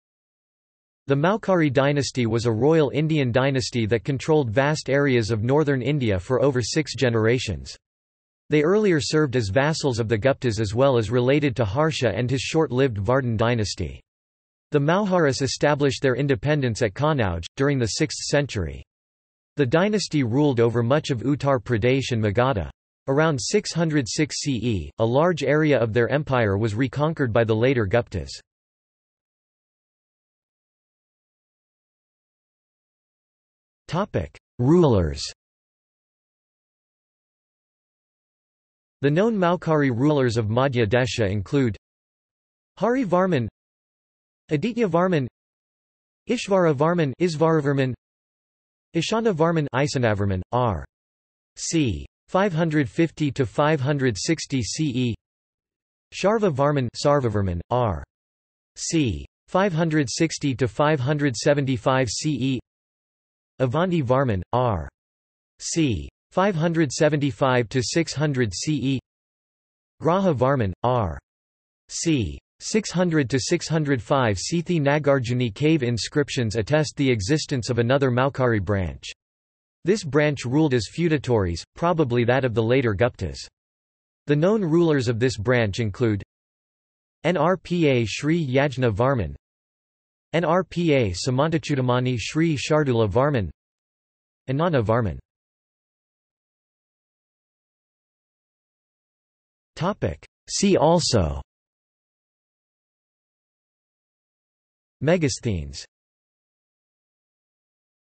Maukhari | Wikipedia audio article
This is an audio version of the Wikipedia Article:
Maukhari

00:00:30 1 Rulers
00:01:36 2 See also

- Socrates

SUMMARY
The Maukhari dynasty was a royal Indian dynasty that controlled vast areas of Northern India for over six generations. They earlier served as vassals of the Guptas as well as related to Harsha and his short-lived Vardhan dynasty. The Maukharis established their independence at Kannauj, during the 6th century. The dynasty ruled over much of Uttar Pradesh and Magadha. Around 606 CE, a large area of their empire was reconquered by the Later Guptas.